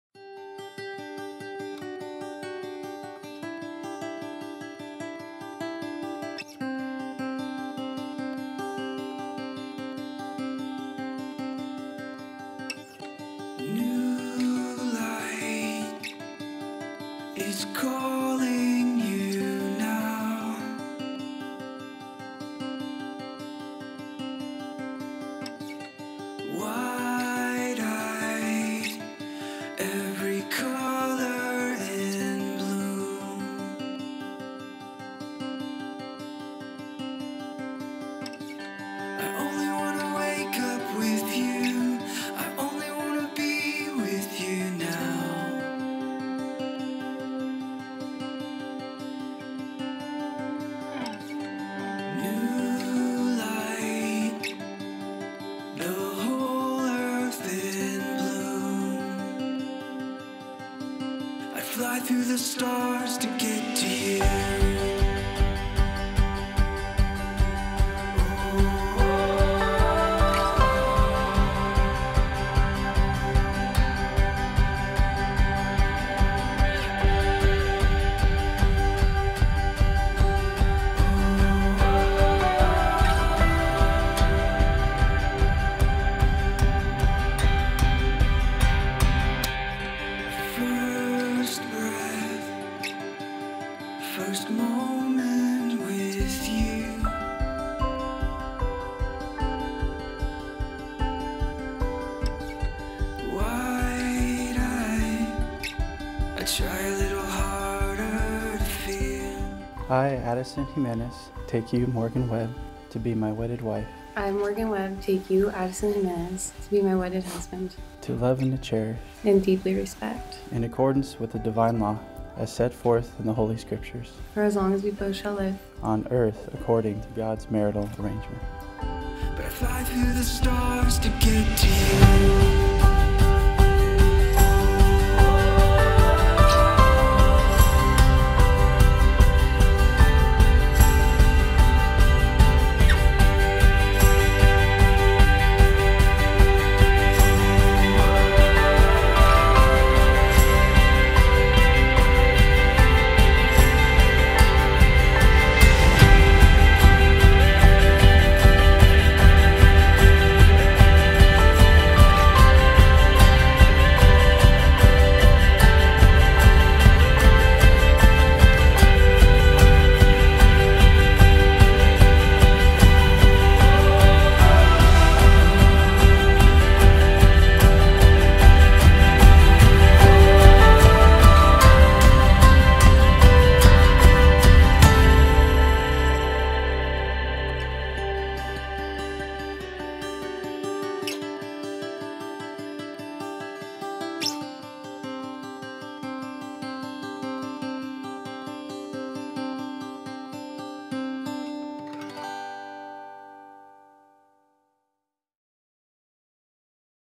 Morgan & Addison | FilmLife Wedding at the A.K. Smiley Library in Redlands, CA
The wedding of two dear friends at the Smiley Library in Redlands, CA.